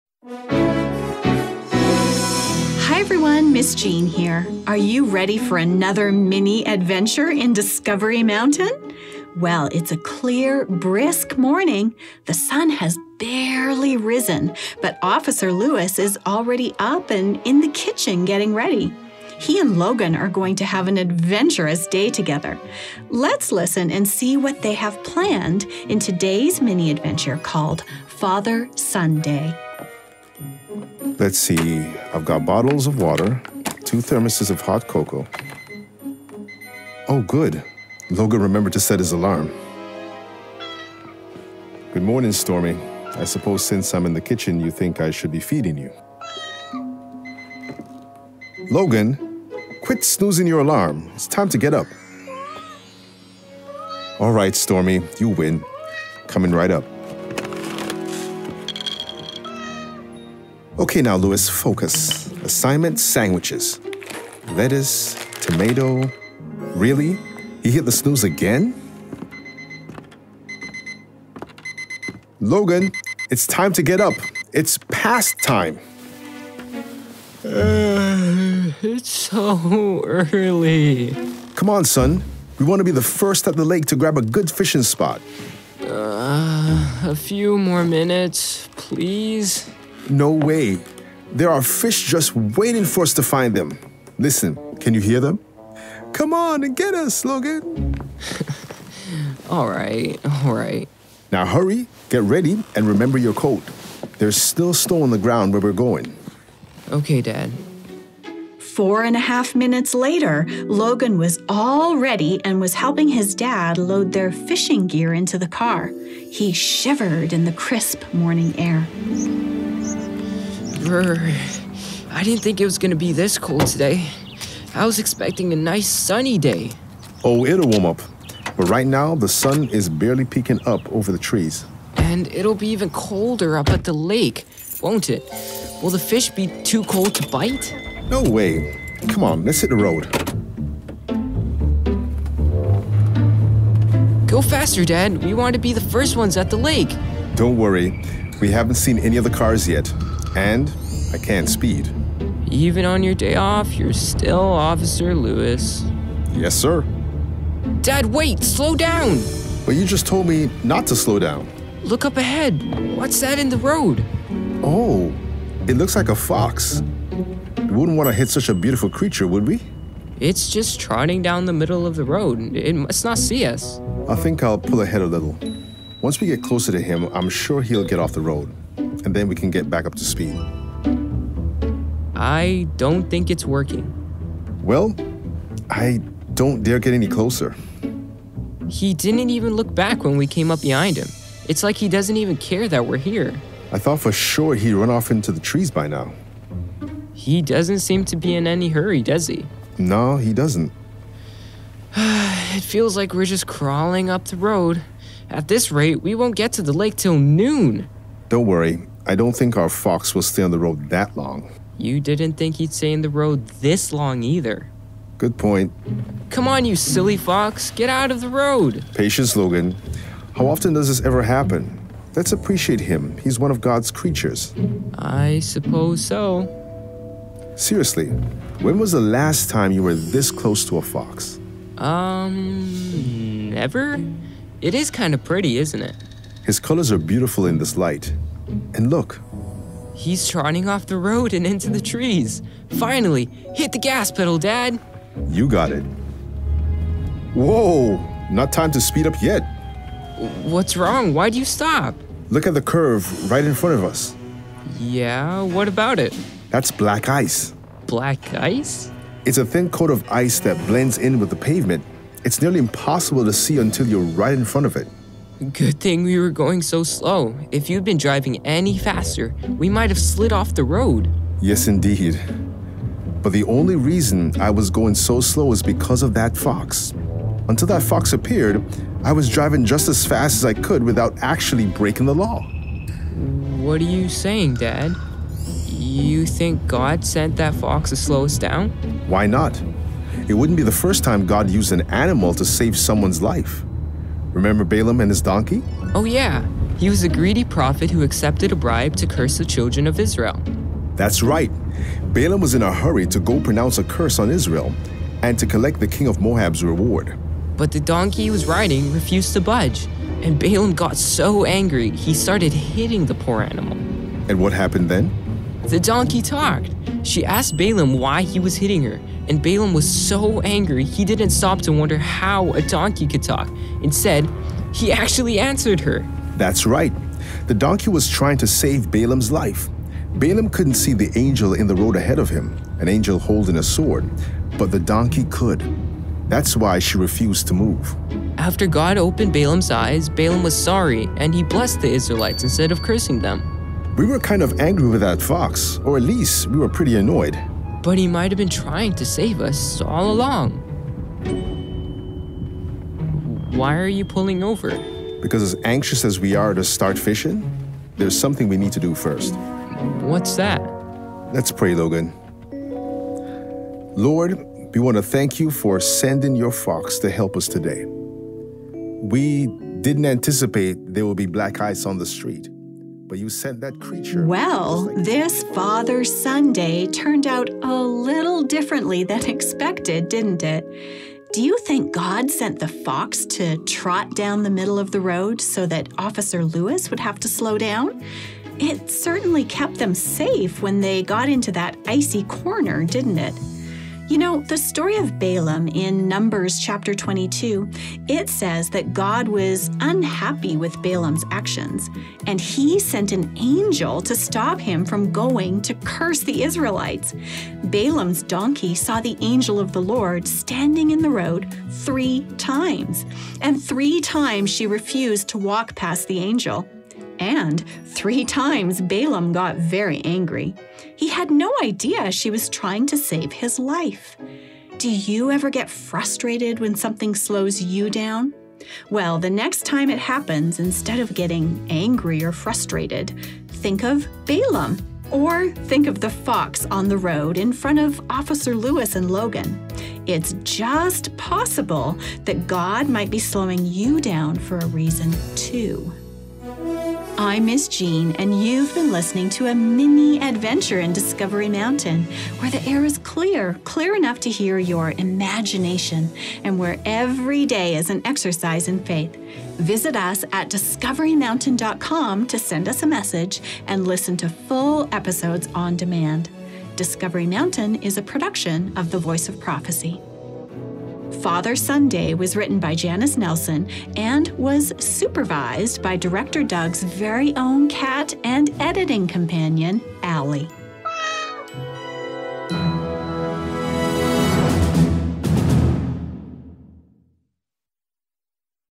Father-Son Day | MINI 2210
A roadblock threatens to stall a father-son adventure for Officer Lewis and Logan. But a barrier can be a blessing—as the biblical Balaam learned.